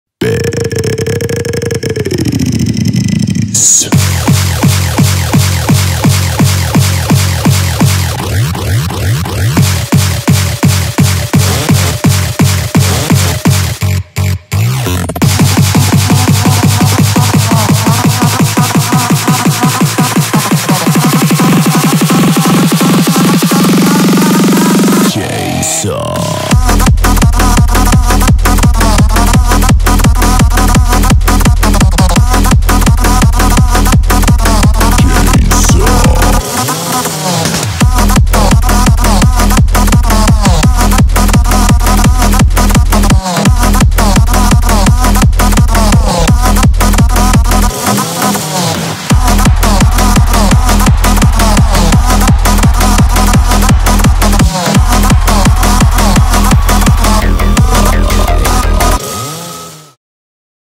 Sounds So Sweet- Vinyl Fantasy Recap Video
Sounds So Sweet invited to UK artist Spyro and Alex Prospect to come down and play some sick music for the people of NY